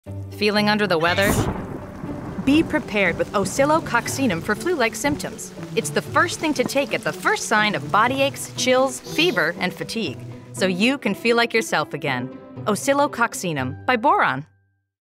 Feeling under the weather? Be prepared with Oscillococcinum
Feeling under the weather?  Be prepared with Oscillococcinum for flu-like symptoms. It's the first thing to take at the first sign of body aches, chills, fever, and fatigue. So you can feel like yourself again.